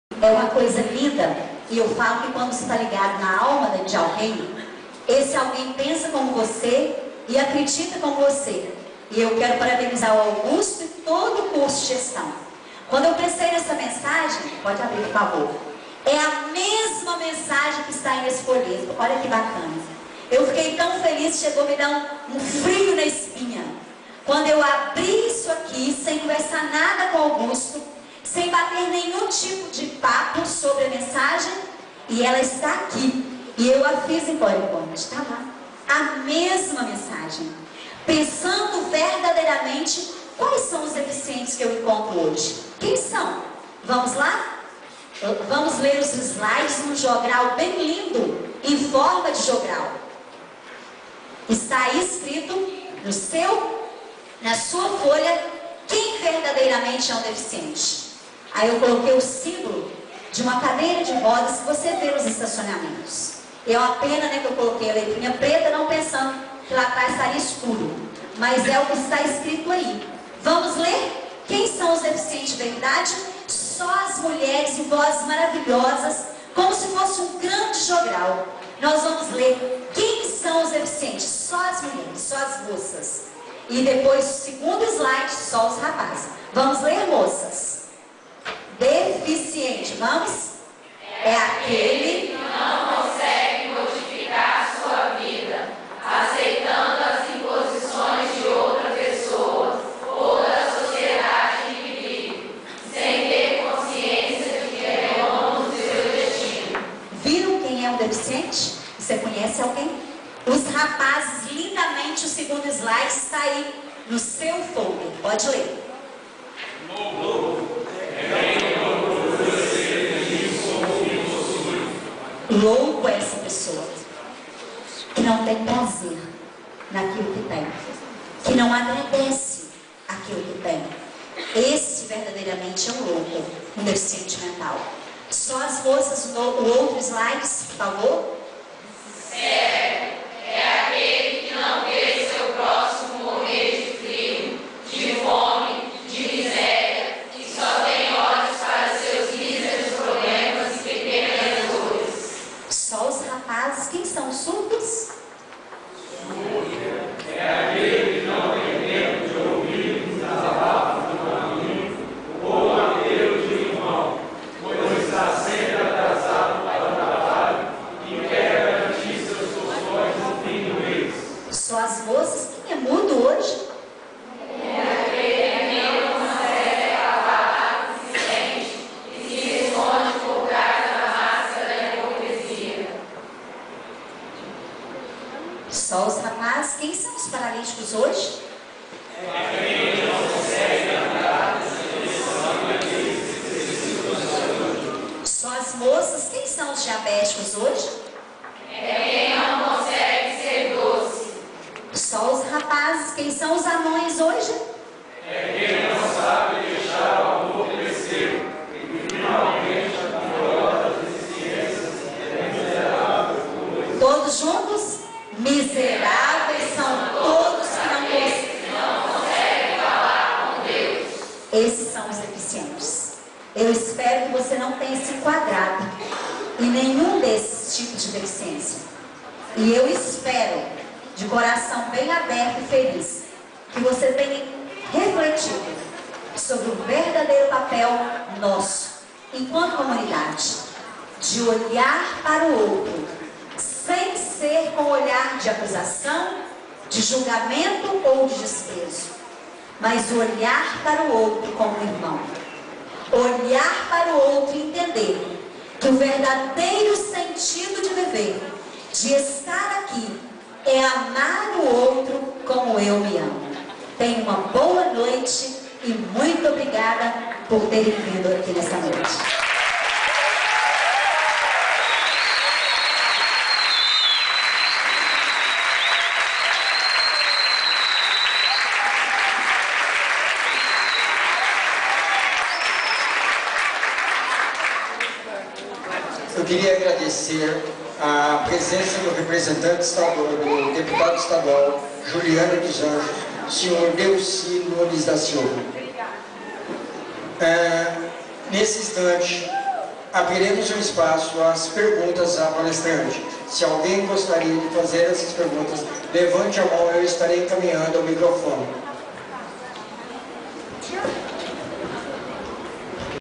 Parte 9 de 12 - Inclusão Social - Ciclo de palestras Em busca do conhecimento
A EEEFM João XXIII deu início hoje, 25 de maio de 2009 à primeira etapa do projeto Em busca co conhecimento, do Curso Técnico em Administração, com tema Inclusão Social.

A primeira palestra foi realizada na Casa da Cultura em Barra de São Francisco e contou com a presença de professores e alunos dos Curso Técnico em Administração.

O ciclo de palestras acontecerá também nos dias 26 e 27 de maio de 2009 com os temas Direito Trabalhista e Ética Empresarial.

Todo o evento foi organizado pelos alunos do curso sob a orientação dos professores, tendo por objetivo levá-los a viverna prática no que consiste a Administração.

Confira todas as fotos do evento